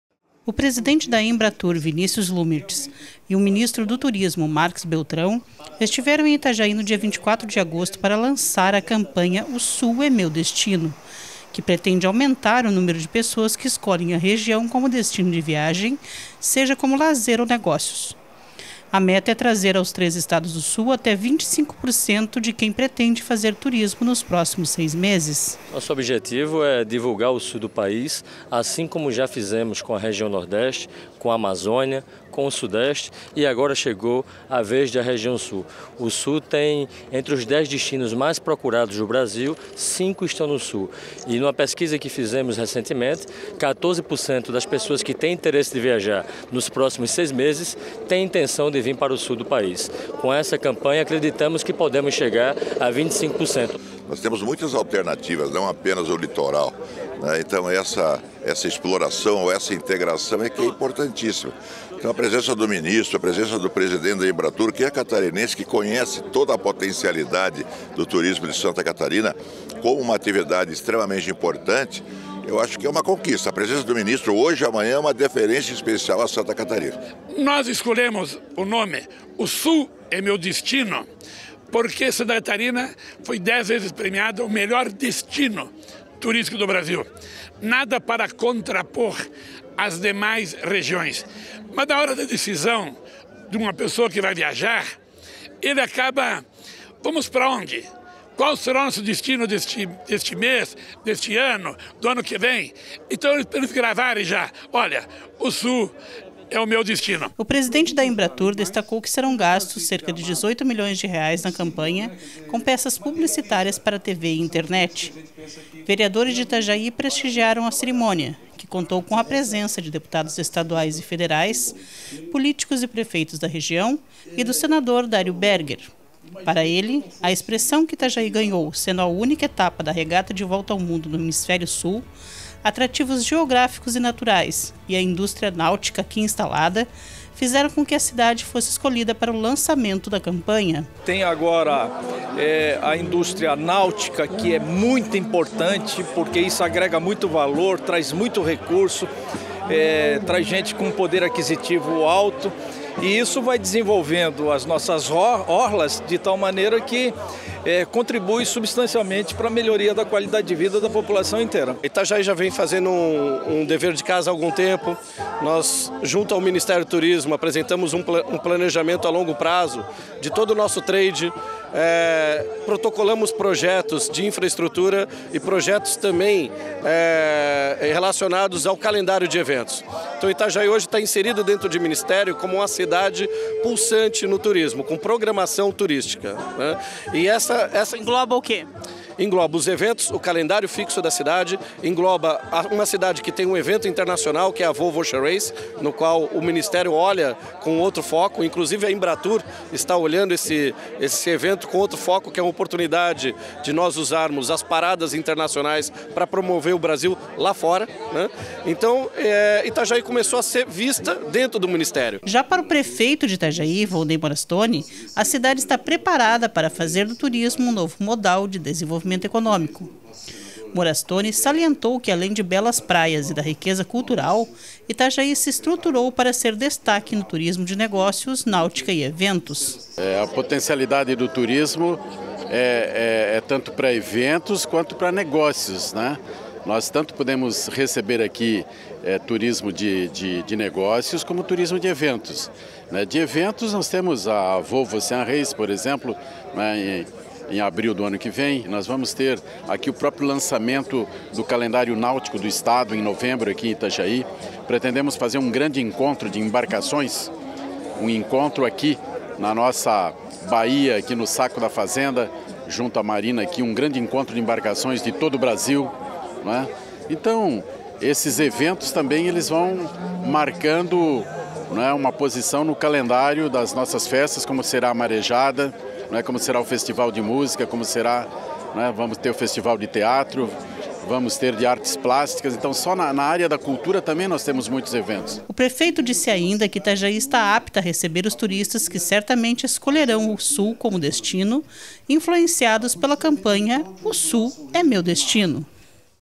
Lançada em Itajaí campanha para promover o sul como destino turístico
A campanha “O SUL É MEU DESTINO”, que pretende promover o turismo na região Sul do Brasil, foi lançada ontem (24/08) em Itajaí. De acordo com o Ministério do Turismo, a meta é fazer com que 25% das pessoas que pretendem viajar nos próximos seis meses, escolham os atrativos catarinenses, gaúchos ou paranaenses. Conforme a Embratur, a escolha de Itajaí para o lançamento da campanha se deve à projeção que a cidade possui, especialmente no turismo náutico.